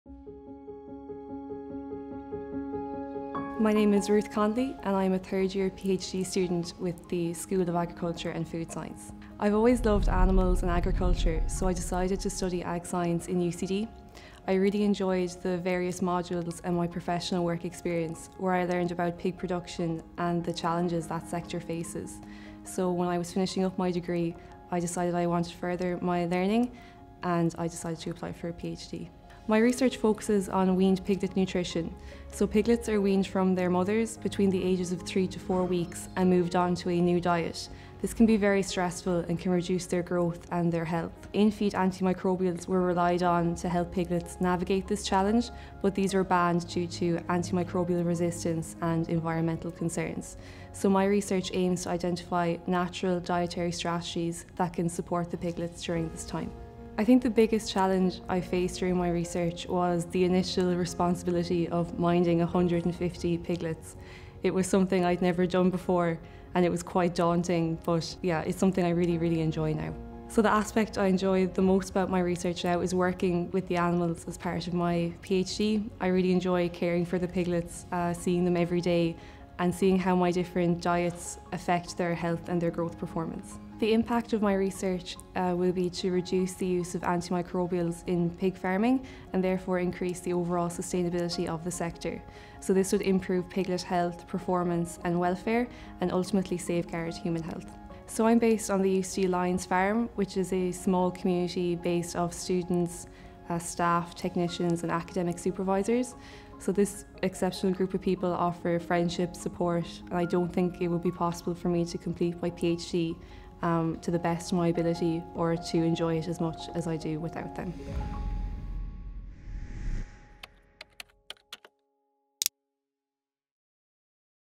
Ruth Connolly: PhD Journey
The UCD Institute of Food and Health are proud to showcase a selection of the research talent from across the UCD campus.

Ruth Connolly's research is: Post-weaning nutritional strategies to increase piglet growth and reduce antimicrobial usage. Ruth tells us about her journey that led her to this area of research and her experience so far.